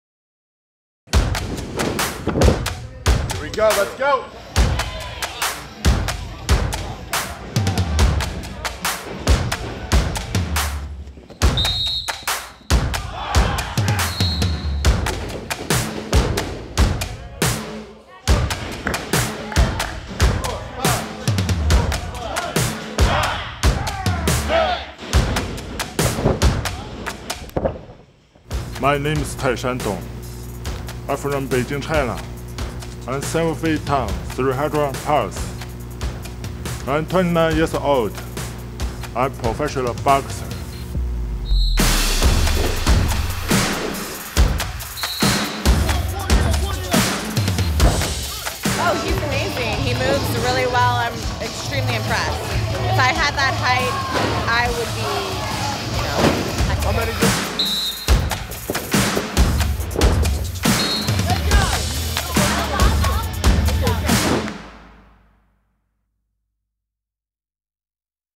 Undefeated boxer "The Great Wall" Taishan Dong tries out for WWE
Seven-foot-tall Taishan Dong of China trades his boxing gloves for wrestling boots during a recent tryout at the WWE Performance Center.